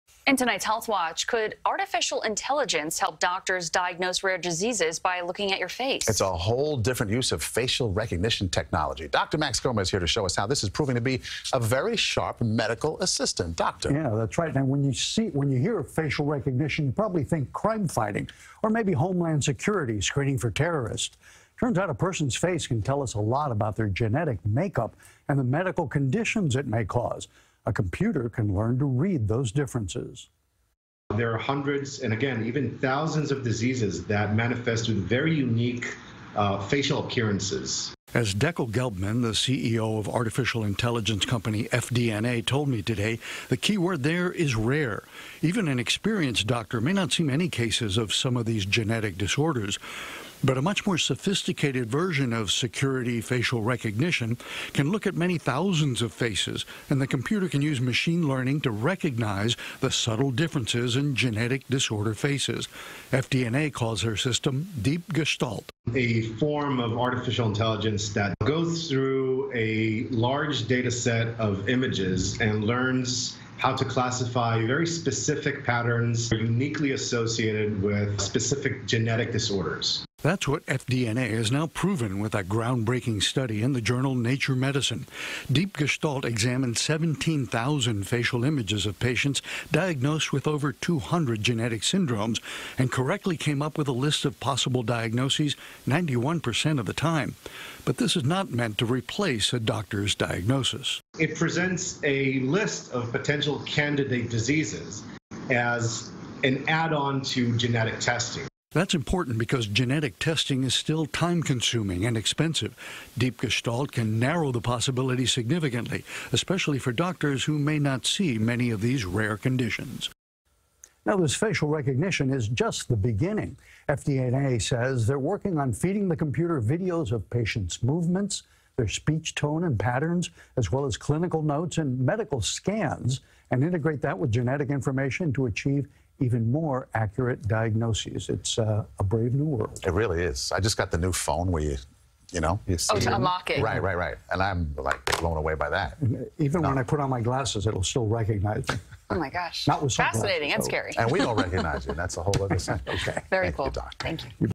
Could Facial Recognition Tech Help Doctors Diagnose Rare Diseases?
CBS2's Dr. Max Gomez shows us how facial recognition is proving to be a very sharp medical assistant.